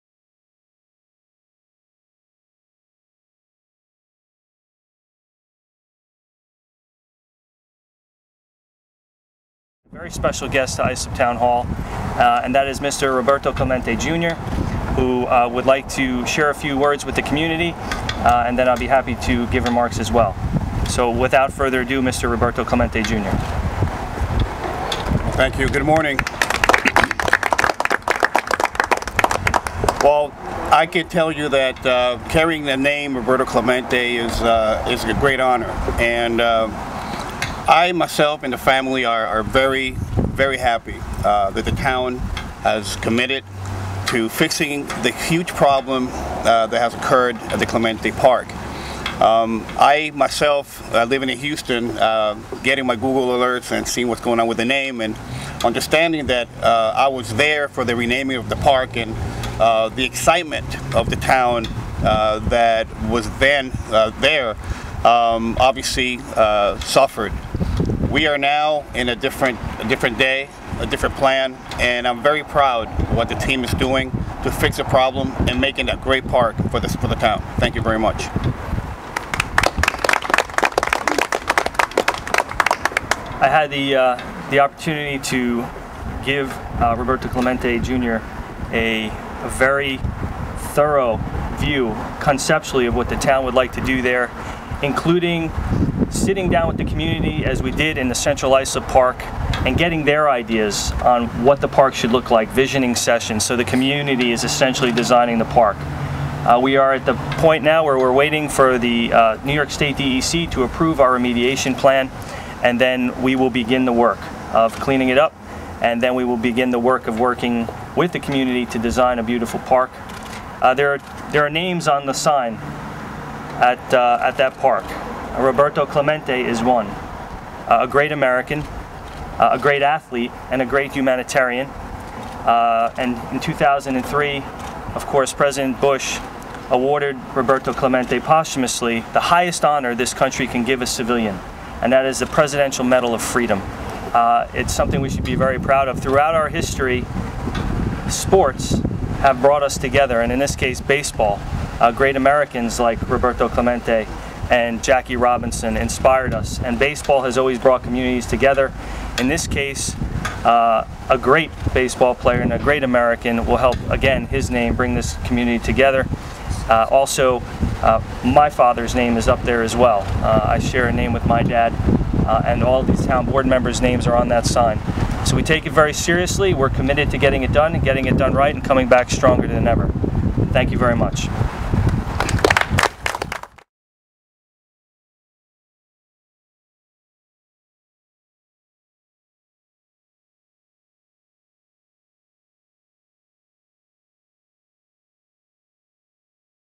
Supervisor Croci meets with Roberto Clemente, Jr.
Roberto Clemente, Jr., son of baseball Hall of Famer Roberto Clemente, met with Islip Supervisor Tom Croci & the Town Board & thanked them for their  ongoing dedication to the remediation of Roberto Clemente Park and its subsequent rebuild and rebirth.